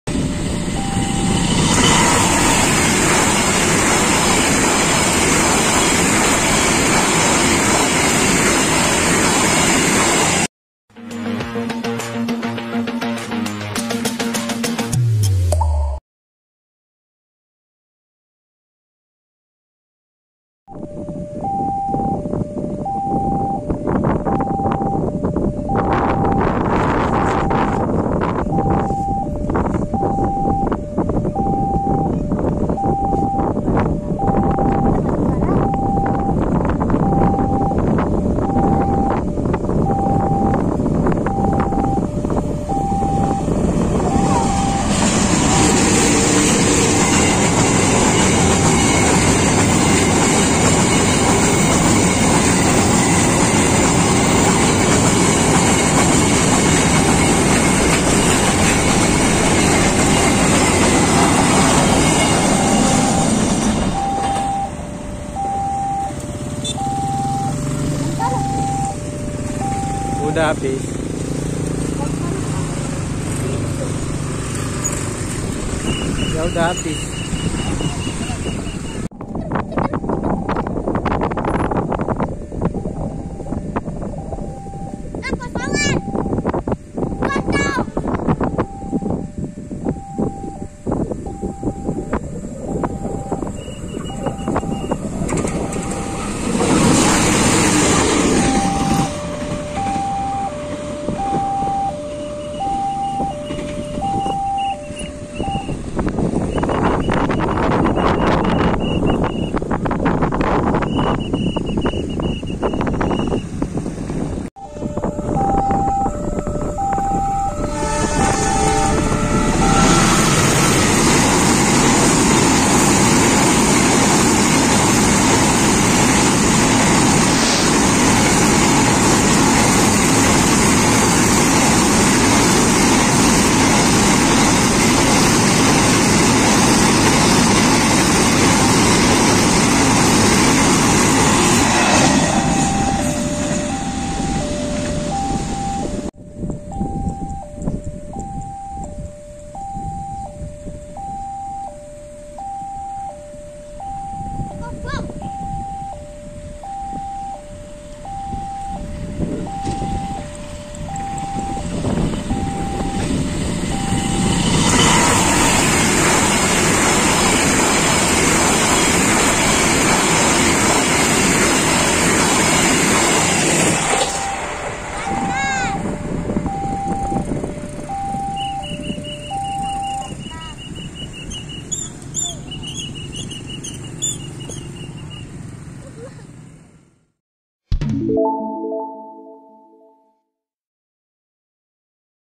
Kompilasi Kereta api 4K Gamel-Sarabau Plered Cirebon
Kereta api melintas di kabupaten Cirebon, tepatnya di perlintasan tanpa palang pintu Gamel-Sarabau Plered.